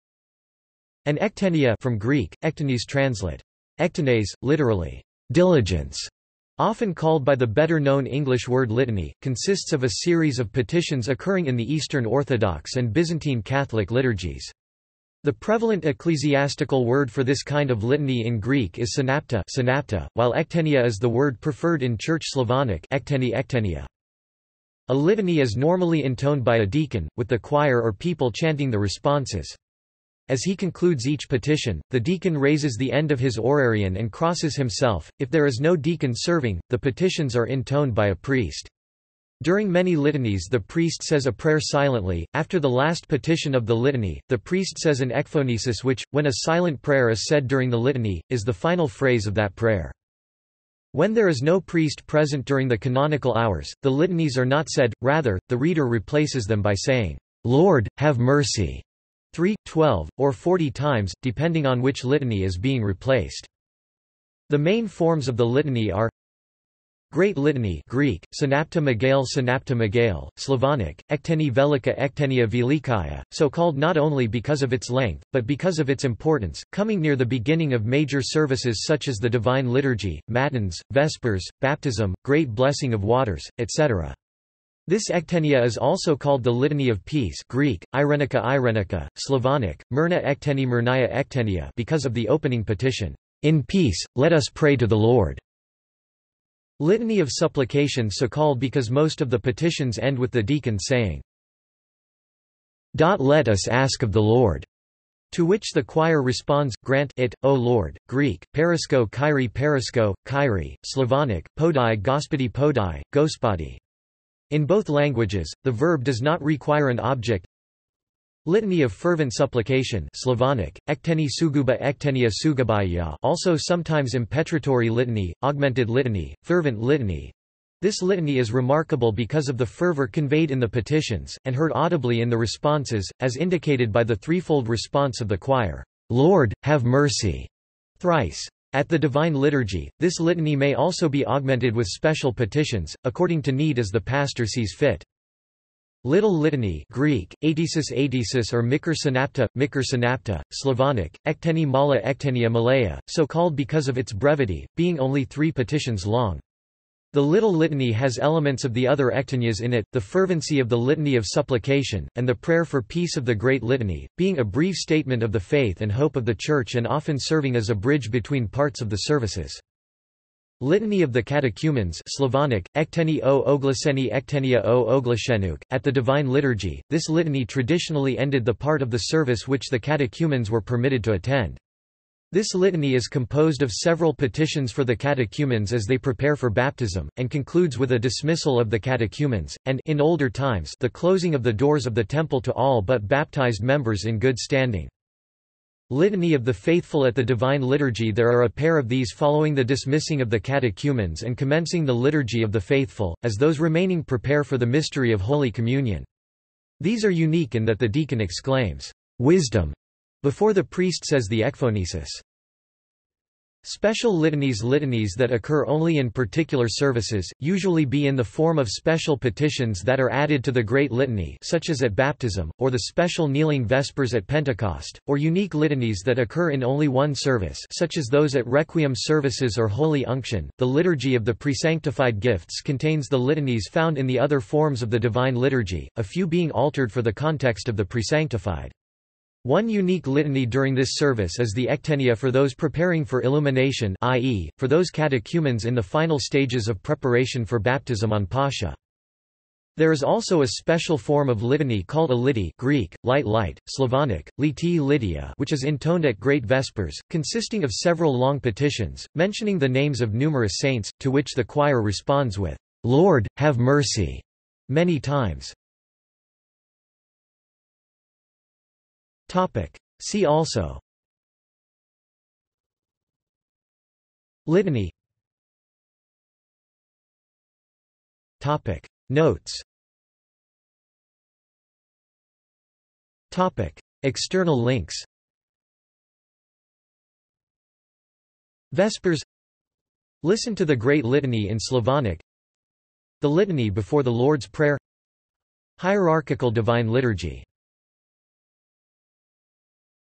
Ectenia | Wikipedia audio article
This is an audio version of the Wikipedia Article:
Ectenia

- Socrates

SUMMARY
An Ektenia (from Greek: ἐκτενής, translit. ektenés; literally, "diligence"), often called by the better known English word litany, consists of a series of petitions occurring in the Eastern Orthodox and Byzantine Catholic liturgies. The prevalent ecclesiastical word for this kind of litany in Greek is Συναπτή (synaptê), while ektenia is the word preferred in Church Slavonic (ектенїѧ ekteniya).
A litany is normally intoned by a deacon, with the choir or people chanting the responses.  As he concludes each petition, the deacon raises the end of his orarion and crosses himself; if there is no deacon serving, the petitions are intoned by a priest. During many litanies the priest says a prayer silently; after the last petition of the litany, the priest says an ecphonesis  which, when a silent prayer is said during the litany, is the final phrase of that prayer.
When there is no priest present during the canonical hours, the litanies are not said; rather, the reader replaces them by saying "Lord, have mercy," three, twelve, or forty times, depending on which litany is being replaced.

The main forms of the litany are:

Great Litany (Greek: Συναπτή μεγάλη/Synaptê Megalê; Slavonic: Ектенїѧ Великаѧ/Ekteniya Velikaya) --so called not only because of its length, but because of its importance, coming near the beginning of major services such as the Divine Liturgy, Matins, Vespers, Baptism, Great Blessing of Waters, etc. This ektenia is also called the Litany of Peace (Greek: Εἰρηνικά/Eirênika; Slavonic: Мирнаѧ Ектенїѧ/Mirnaya Ekteniya) because of the opening petition: "In peace, let us pray to the Lord."
Litany of Supplication--so called because most of the petitions end with the deacon saying, "...let us ask of the Lord," to which the choir responds, "Grant [it], O Lord.” (Greek: Παράσκου, Κύριε/Paraskou, Kyrie; Slavonic: Подаи, Господи/Podai, Ghospodi. In both languages, the verb does not require an object.)
Litany of Fervent Supplication (Slavonic: Ектенїѧ Сугубаѧ/Ekteniya Sugubaya)  also sometimes Impetratory Litany, Augmented Litany, Fervent Litany—this litany is remarkable because of the fervor conveyed in the petitions, and heard audibly in the responses, as indicated by the threefold response of the choir, "Lord, have mercy" (thrice). At the divine liturgy, this litany may also be augmented with special petitions, according to need as the pastor sees fit.
Little Litany (Greek: Αἴτησις/Aitêsis or Μικρὴ Συναπτή/Mikrê Synaptê; Slavonic: Ектенїѧ Малаѧ/Ektenia Malaya) --so called because of its brevity, being only three petitions long. The Little Litany has elements of the other ektenias in it: the fervency of the Litany of Supplication, and the prayer for peace of the Great Litany, being a brief statement of the faith and hope of the church and often serving as a bridge between parts of the services.
Litany of the Catechumens (Slavonic: Ектенїѧ о Оглашенныхъ/Ekteniya o Oglashennuikh) --at the Divine Liturgy, this litany traditionally ended the part of the service which the catechumens were permitted to attend. This litany is composed of several petitions for the catechumens as they prepare for baptism, and concludes with a dismissal of the catechumens, and (in older times) the closing of the doors of the temple to all but baptized members in good standing.
Litany of the Faithful--at the divine liturgy there are a pair of these following the dismissing of the catechumens and commencing the Liturgy of the Faithful, as those remaining prepare for the mystery of Holy Communion. These are unique in that the deacon exclaims, "Wisdom!" before the priest says the ecphonesis.
Special Litanies--litanies that occur only in particular services, usually be in the form of special petitions that are added to the Great Litan ...